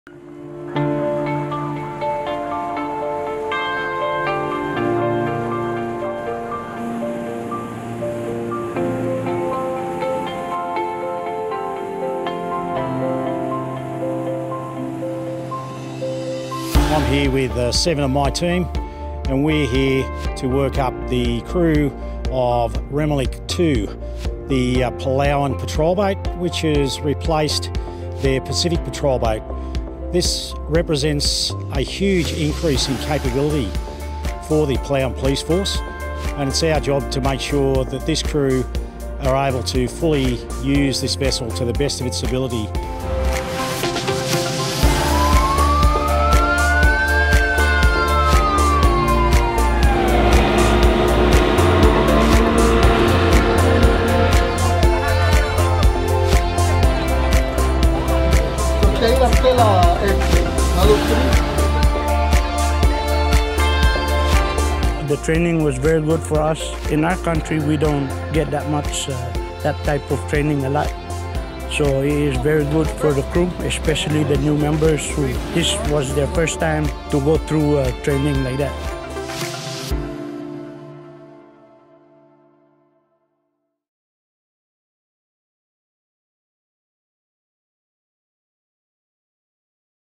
Palau's PSS President HI Remeliik II put through its paces
The Guardian Class Patrol Boat delivered to Pacific partner nation of Palau was recently put through its paces by Navy’s Sea Training Group during a busy schedule of daily runnings out of Fleet Base West, Western Australia.

PSS President HI Remeliik II is the seventh Guardian Class Patrol Boat to be delivered under the Pacific Maritime Security Program.